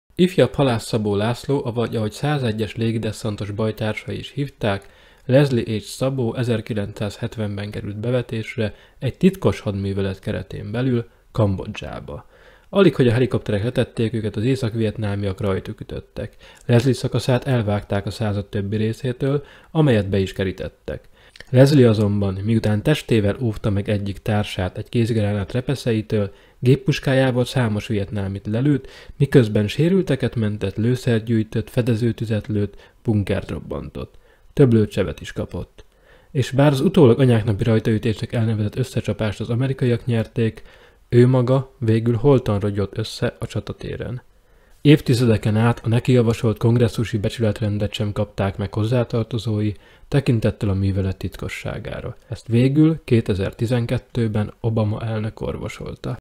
Halász Szabó László – a magyar Rambo | Történelem egy percben
Ifjabb Halász Szabó László azon három magyar származású amerikai katona egyike, akik a vietnámi háború alatt megkapták a legmagasabb szintű amerikai katonai kitüntetést, a Kongresszus Becsületrendet.
Aki kíváncsi a többi amerikai zászló alatt harcoló magyar történetére is, avagy a Magyar Néphadsereg 1973 és '75 közötti dél-vietnámi békefenntartó missziójára, annak ajánlom figyelmébe a Magyarok a vietnámi háborúban címmel feltöltött, nagyobb lélegzetvételű videómat is: